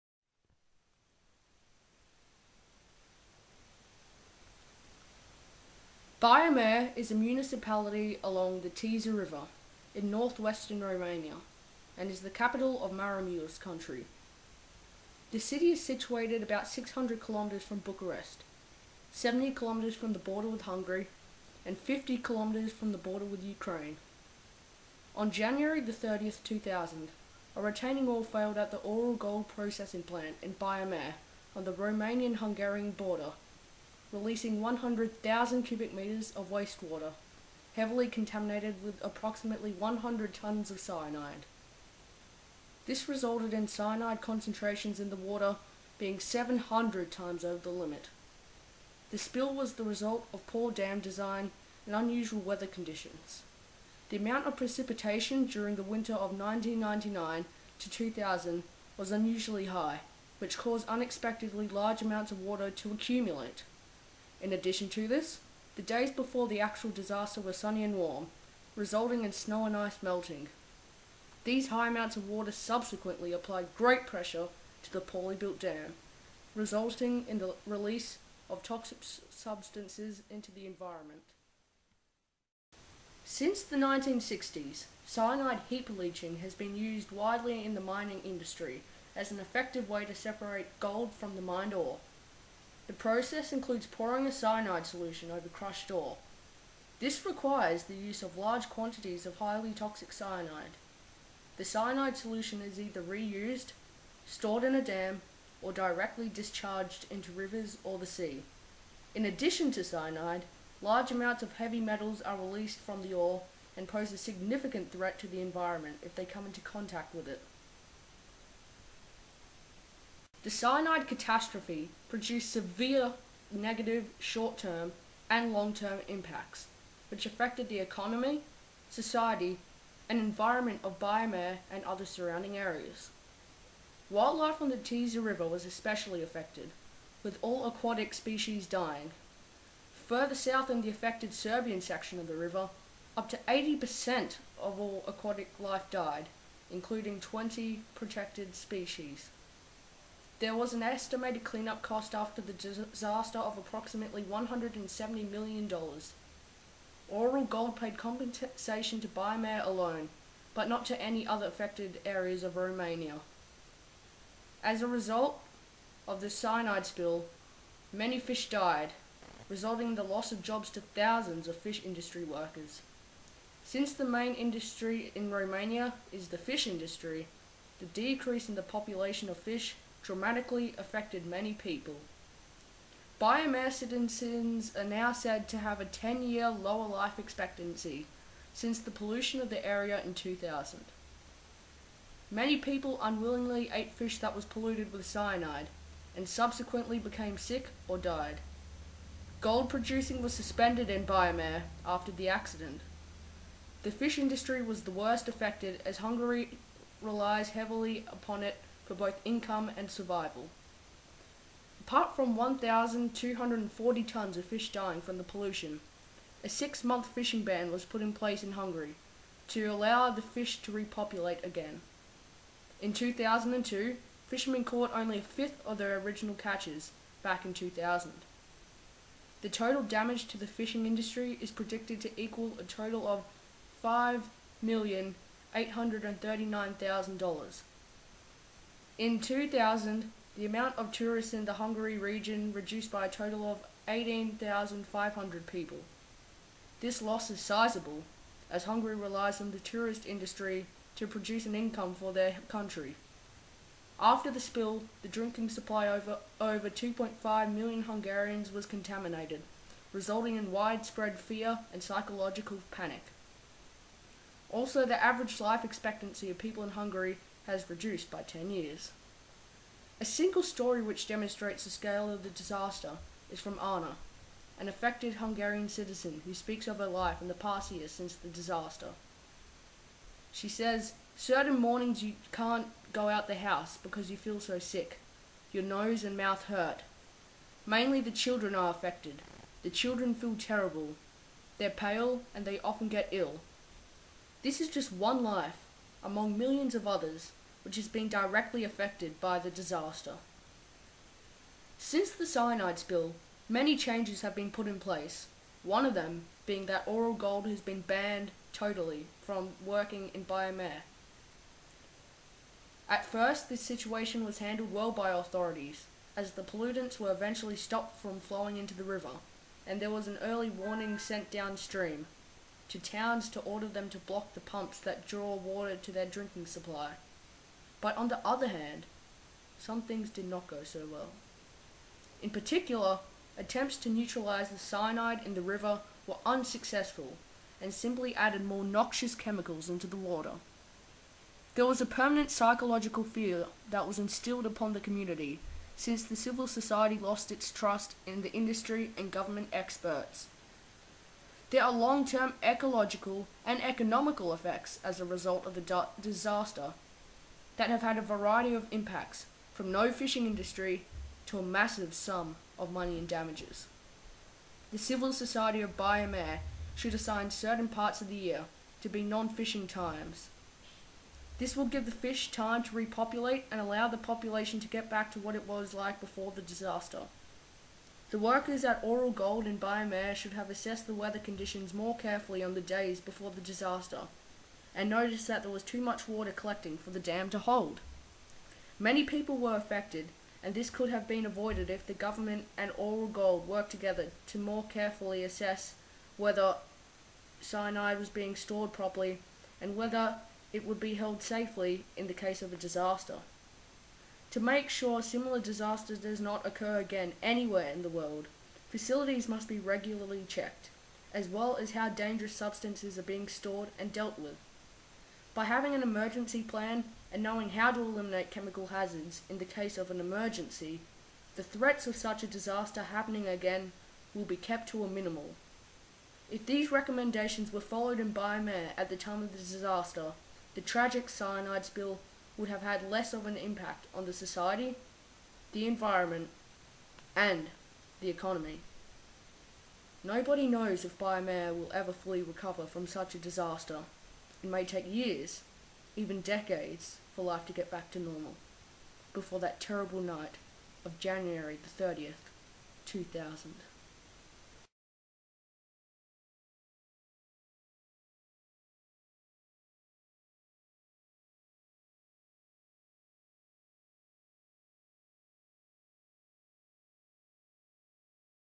Baia Mare Cyanide Spill
This is my high school Geography assignment, focusing on the process and consequences of the Baia Mare Cyanide Spill.